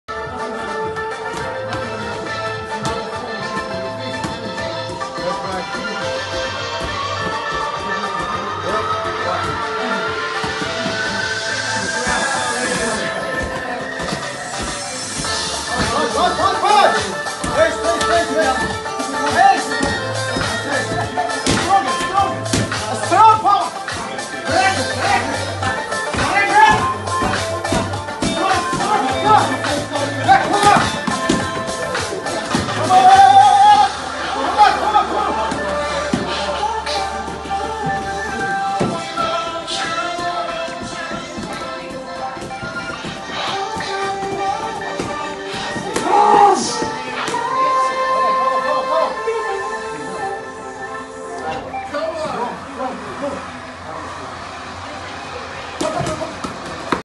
Punch Block Defense to Attack from Guard position
watch and learn how to change Punch Block Defense to Attack from Guard position